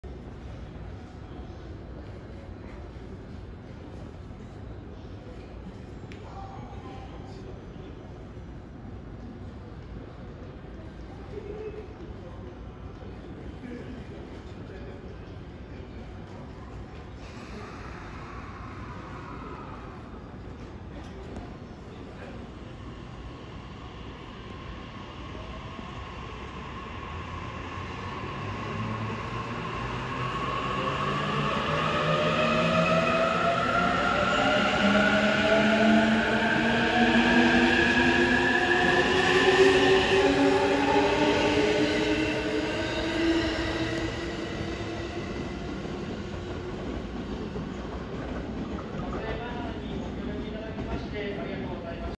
【NEX中央線乗り入れ運用ラストラン】E259系ｸﾗNe005編成（新塗装）が特急成田エクスプレス52号八王子行きとして静かに三鷹駅4番線を発車するシーン（2252M）2024.3.15.22:29
さよなら👋ホームドア関係と需要問題で撤退となりました。

てか特急列車のラストランで鈍行で先回りできるタイプってかなり久々では